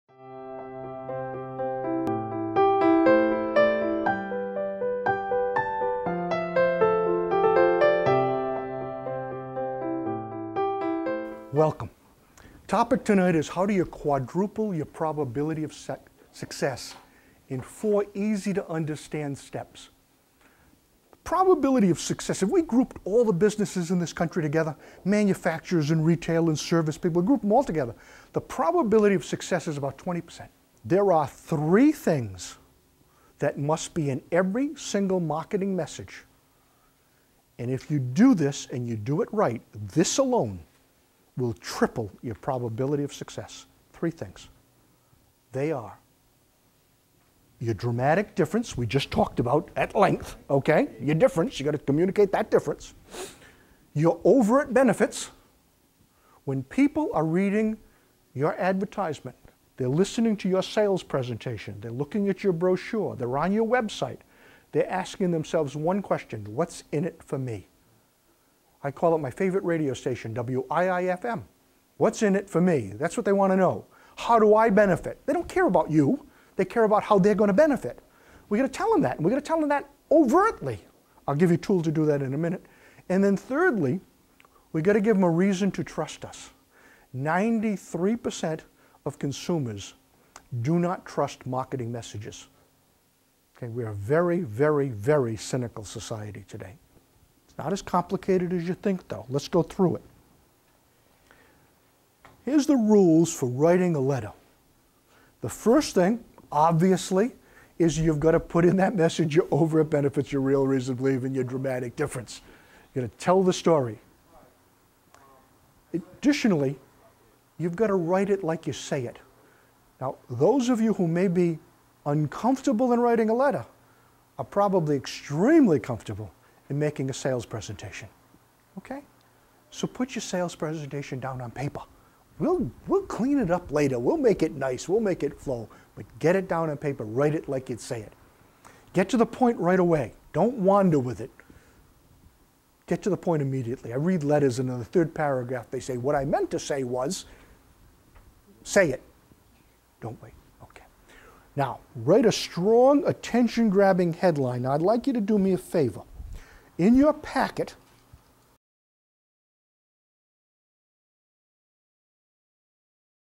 Dave Nash Clip.mov
This is a piece of a seminar that Dave gave. He supplied video d zine with his slides and we edited them in.